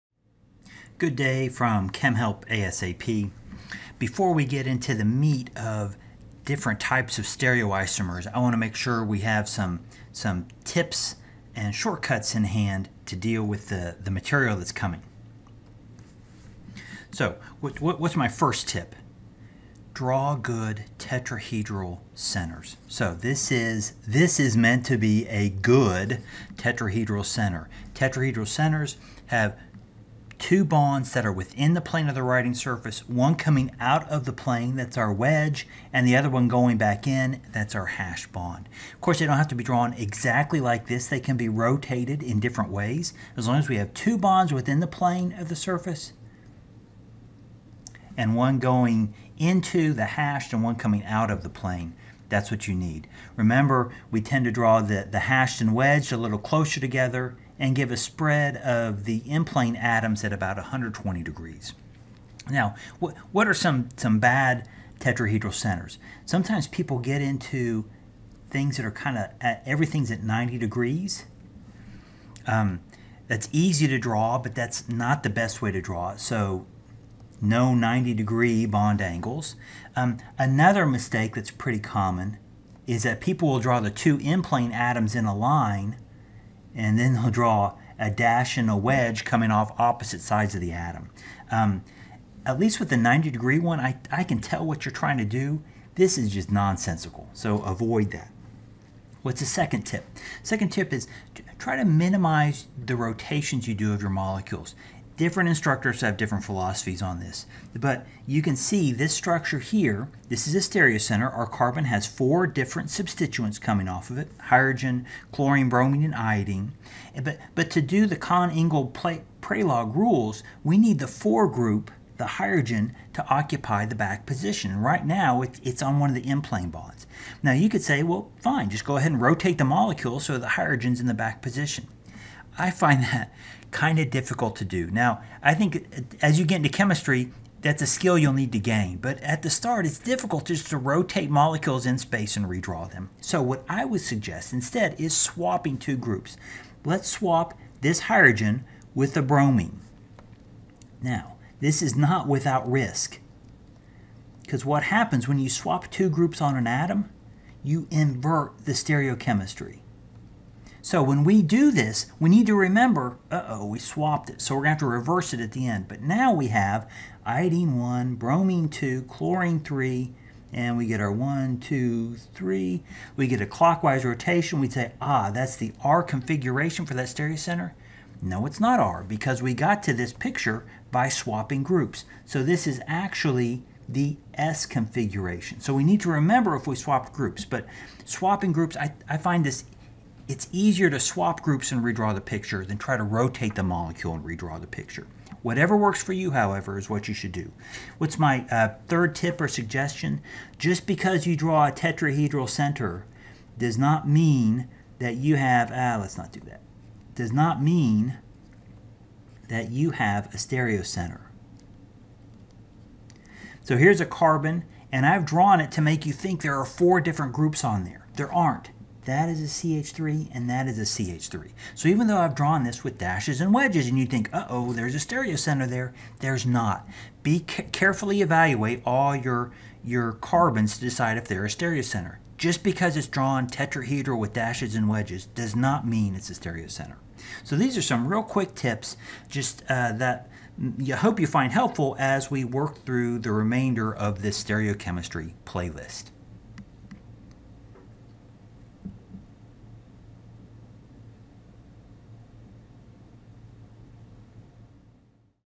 suggestions for drawing stereocenters
Some tips for stereocenters.  (1) Draw proper sp3 hybridized carbons.  Two bonds should be in the plane of the writing surface.  One bond should be wedged.  The last bond should be hashed.  (2) In order to get the #4 group in the back position for stereochemical assignment, swap the position of two groups instead of rotating the molecule.  Remember that swapping two groups will reverse the configuration, so the correct answer will be the opposite of what you determine.  (3) Not all sp3 hybridized carbons are stereocenters.  To be a stereocenter, a carbon must have four different groups.